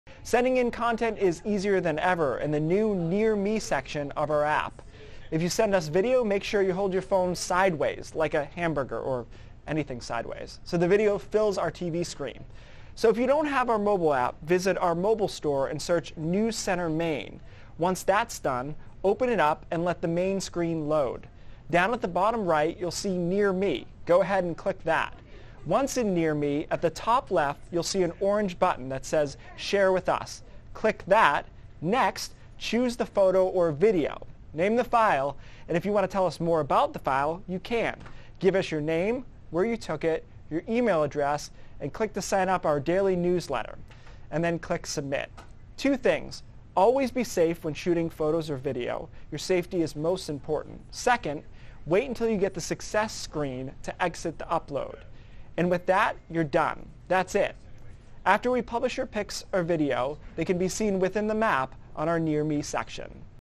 How to use the Near ME tool on the NEWS CENTER Maine mobile app
It's easier than ever to send us photos and videos.